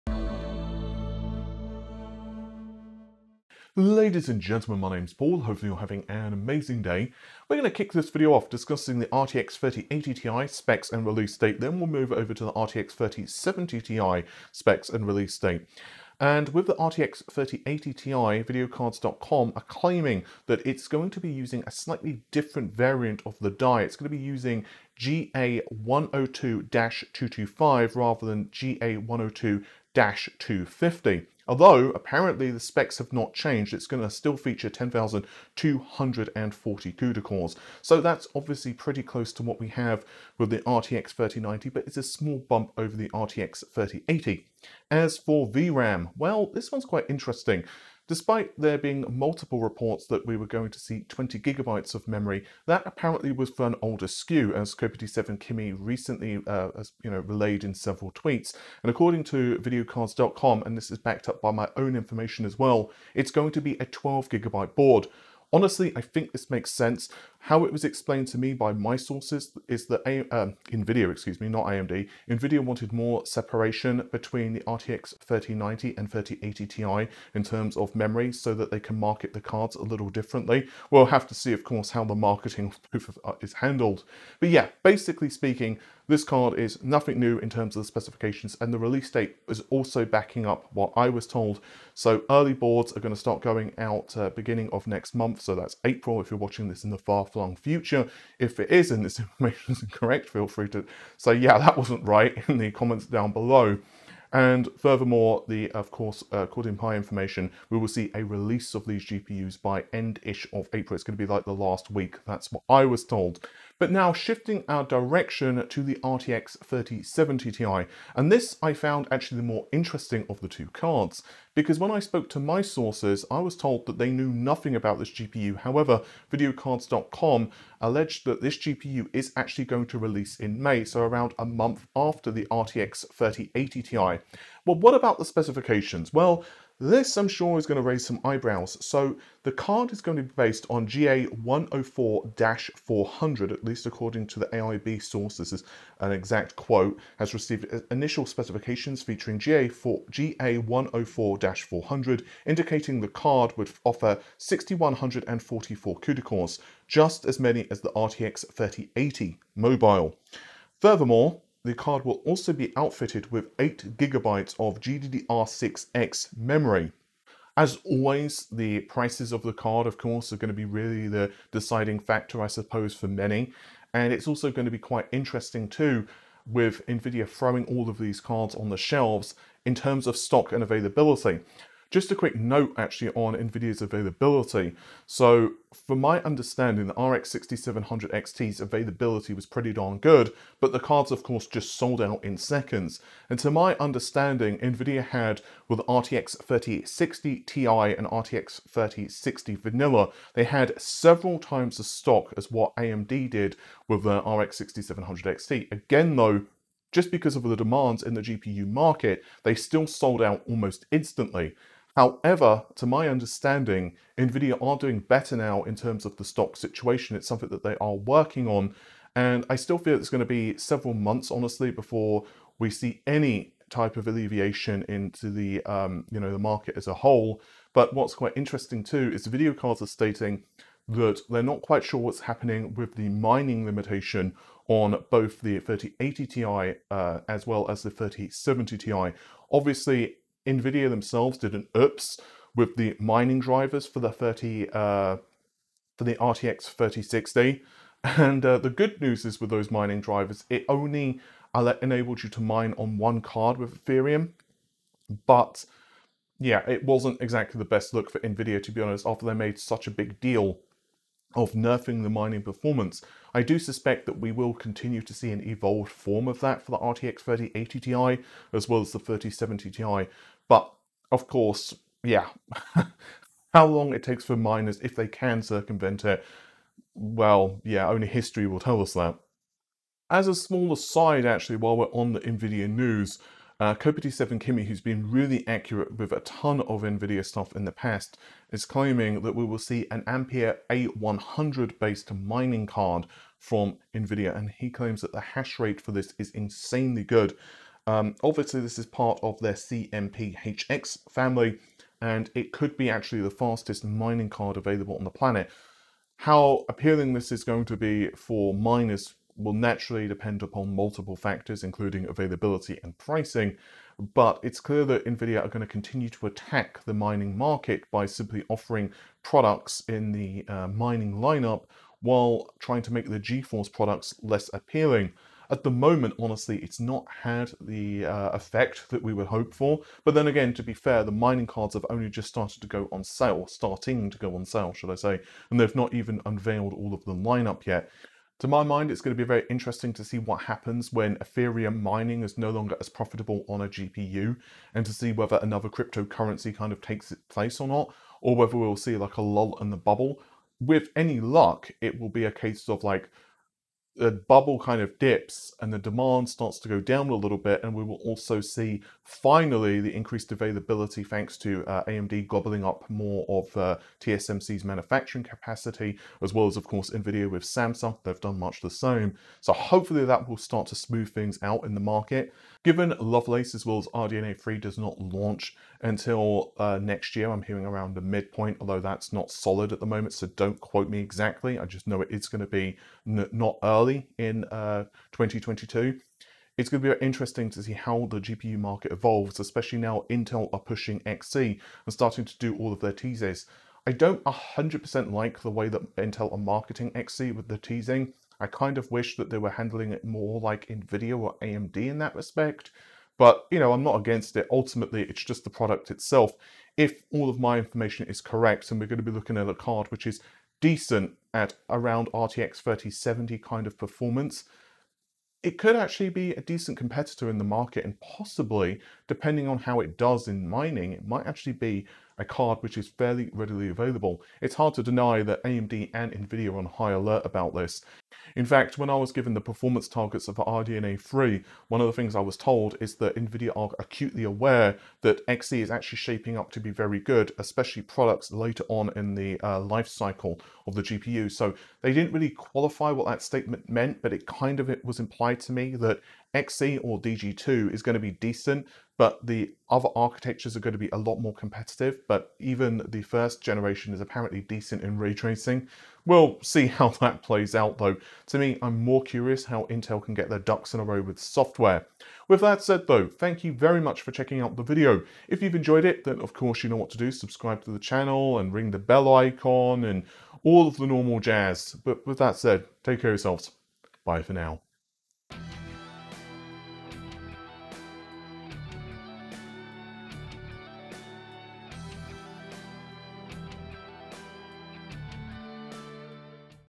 RTX 3070 Ti 8GB Is Real! - Specs | RTX 3080 Ti Specs & Release Date Kills Nvidia's RTX 3090
Nvidia's GeForce RTX 30780 Ti is real and is outfitted with the same number of CUDA cores as the mobile RTX 3080, and also 8GB of GDDR6X memory. The GeForce RTX 3070 Ti 8GB will launch in May.

Meanwhile, the Nvidia RTX 3080 Ti is filled with 12GB of GDDR6X memory, and 10240CUDA cores. The RTX 3080 Ti also releases in April according to both VideoCardz and my own sources, with the GPU competing very well to the RTX 3090 and of course, AMD's own Radeon RX 6900 XT.